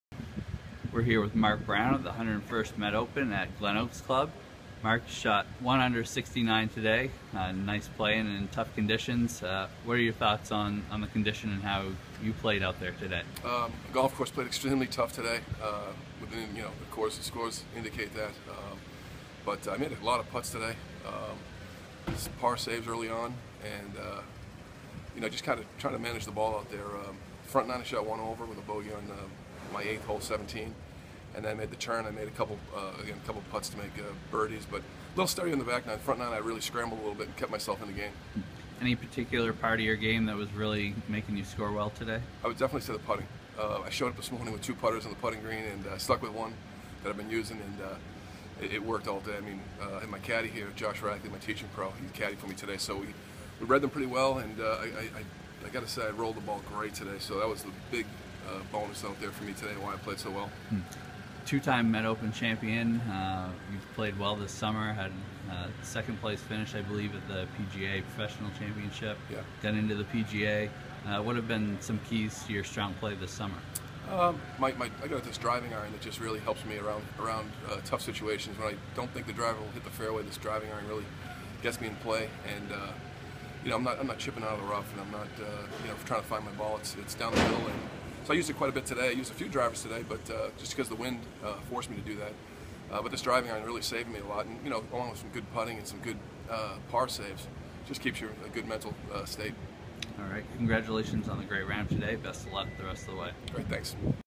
Mark Brown cards 1-under 69 to open 101st Met Open
Two-time Met Open champion Mark Brown shoots 1-under 69 at Glen Oaks Club to begin the 101st Met Open.